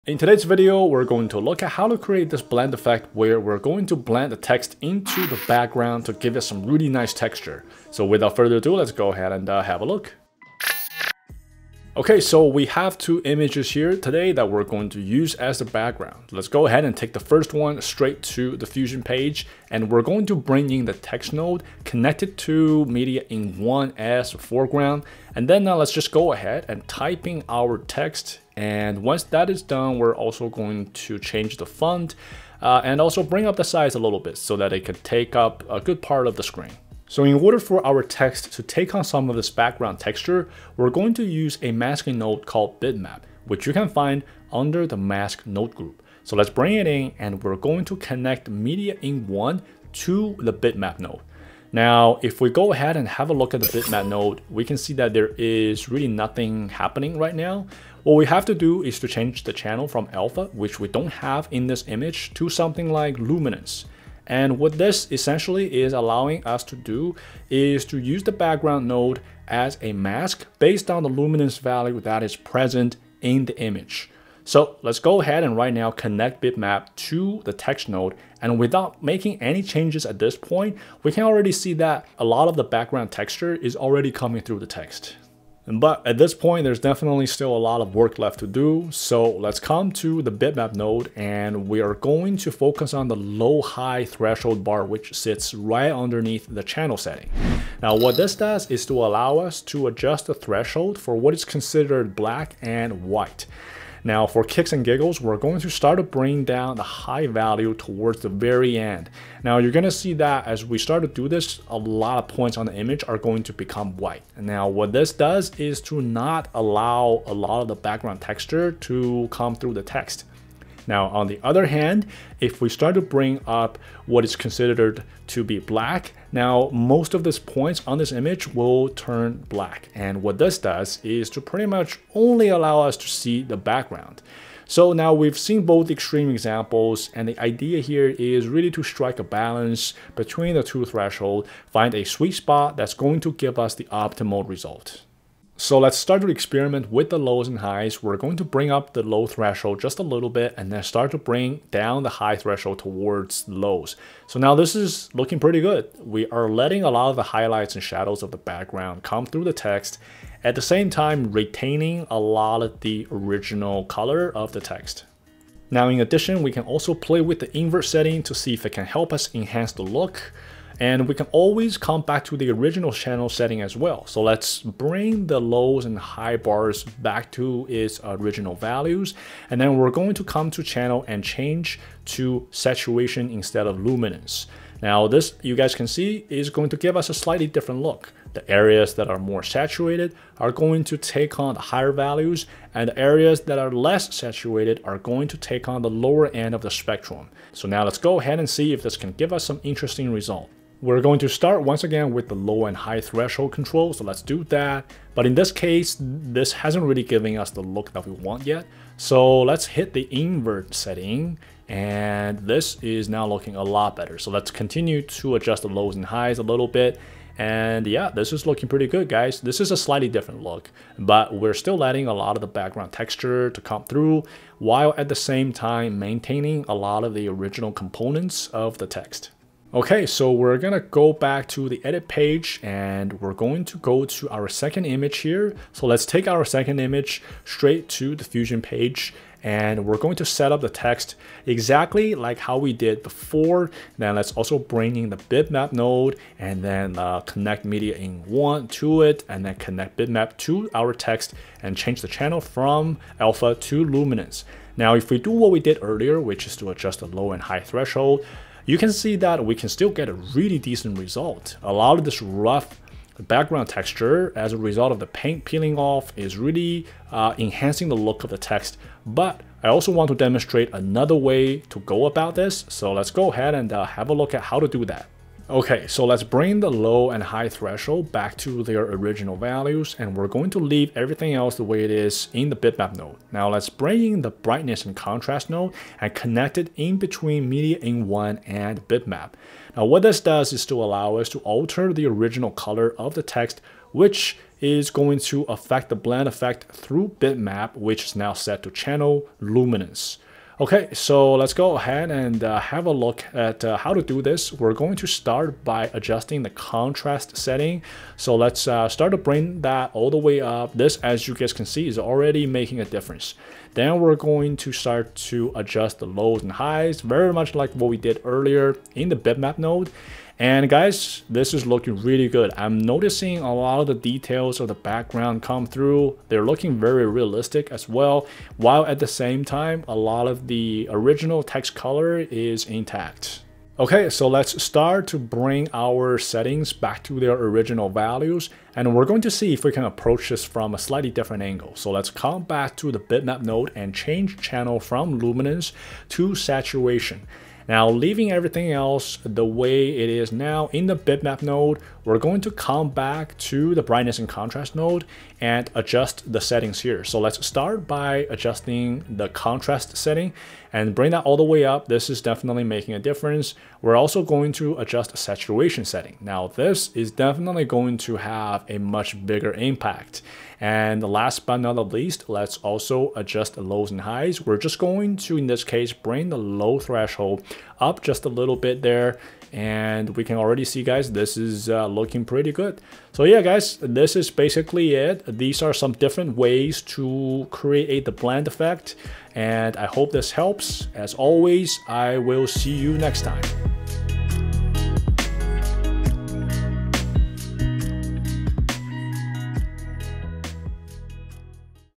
How to Blend Text with Background Photo - Blend Text Effect Tutorial in DaVinci Resolve
In today's tutorial, we will look at how to blend text into the texture of a background photo for a realistic effect. Our goal is to make the text look like it was painted on the background in the first place or it was part of that original photo rather than something added on in post production.

You can also get super creative with effect using not just text but also people, you can use this effect not just in video production but also in YouTube thumbnail, commercial poster, etc. So, hope you enjoy!

Key Takeaways:
00:00 Preview of Effect and Setup
00:38 Blend Text
02:45 Food for Thought
03:55 Another Example
04:30 Enhance the Effect